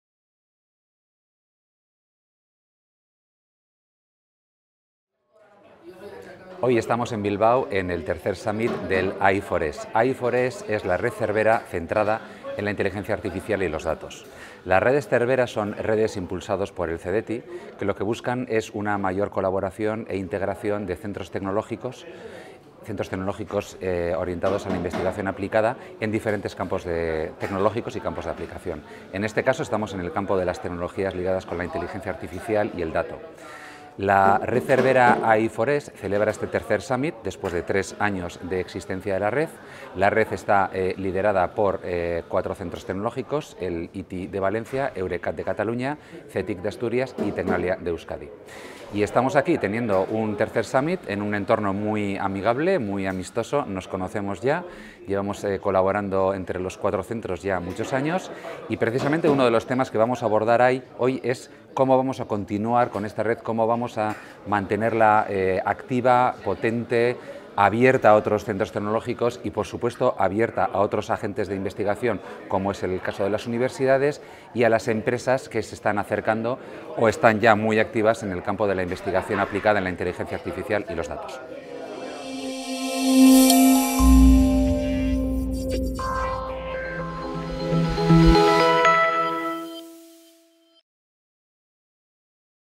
Joseba Laka - red de excelencia en tecnologías habilitadoras basadas en el dato
Joseba Laka ha presentado la red de excelencia en tecnologías habilitadoras basadas en el dato, AI4ES. En el tercer AI4ES SUMMIT celebrado el 30 de noviembre en Bilbao

El objetivo de AI4ES es potenciar la economía basada en datos e inteligencia artificial a nivel nacional, facilitando la transferencia de capacidades, resultados y tecnologías al tejido productivo, proporcionando soluciones y servicios innovadores que requieran de acceso seguro y confiable a datos, procesamiento eficiente, análisis avanzado, aprendizaje automático, e inteligencia artificial.

Para ello, la red AI4ES agrega y armoniza las capacidades, infraestructuras y actividades de I+D en estas tecnologías por parte de cuatro centros tecnológicos españoles de referencia a nivel nacional e internacional: el Instituto Tecnológico de Informática (ITI, coordinador de la red), Fundación CTIC Centro Tecnológico, Eurecat, y TECNALIA.